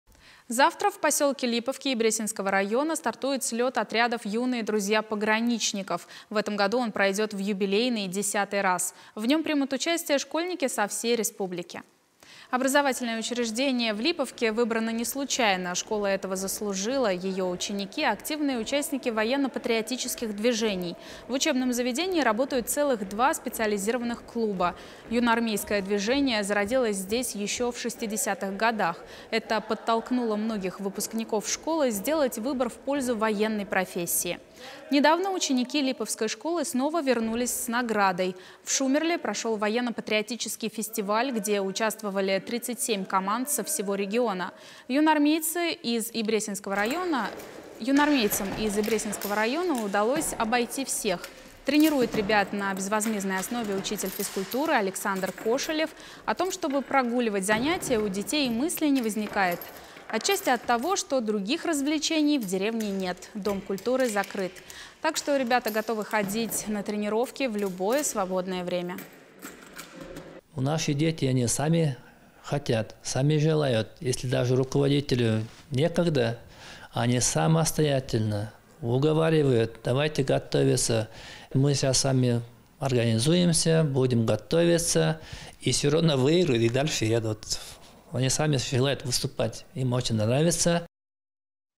В Ибресинском районе стартует юбилейный слет отрядов
Завтра в поселке Липовке Ибресинского района стартует слет отрядов "Юные друзья пограничников". В этом году он пройдет в юбилейный - 10-й раз. В нем примут участие школьники со всей республики.